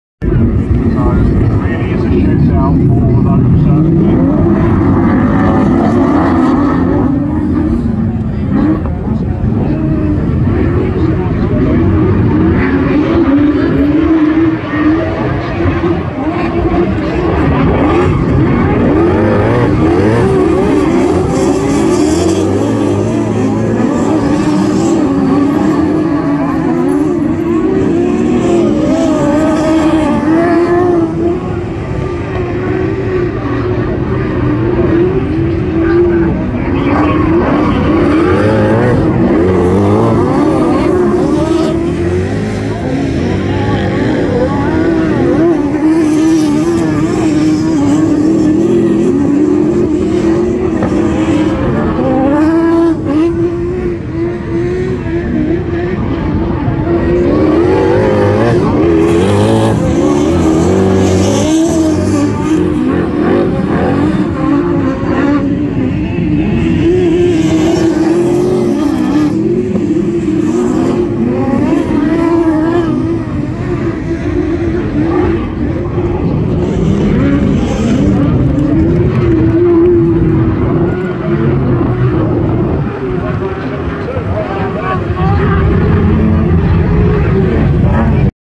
British Autograss Series Rd 4 Class 7 8 Border Counties Day 1 14th September 2024
British Autograss Series Round 4 held by Border Counties Autograss Club day 1 14th September 2024